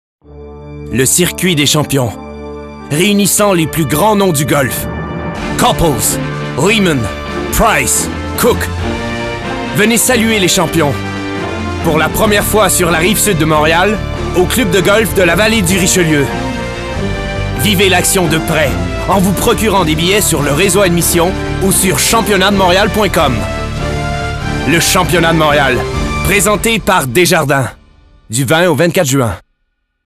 Championnat de Montréal 2012 - Pub télé (Billets)
Publicité télévision de 30 secondes faite pour le Championnat de Montréal 2012, un événement du Circuit des Champions de la PGA.  En vedette : Fred Couples, Tom Lehman, Nick Price et John Cook.

Du 18 au 24 juin 2012 au Club de golf de la Vallée du Richelieu.